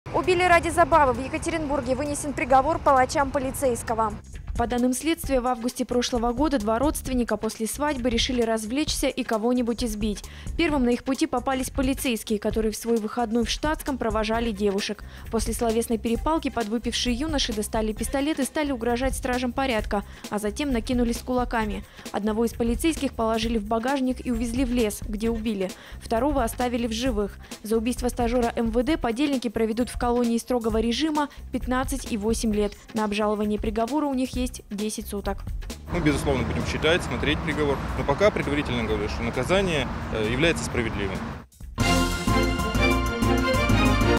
Убийство полицейского ради забавы обернулось 23 годами тюрьмы
В Екатеринбурге вынесен приговор палачам полицейского. По данным следствия, в августе прошлого года два родственника после свадьбы решили развлечься и кого-нибудь избить.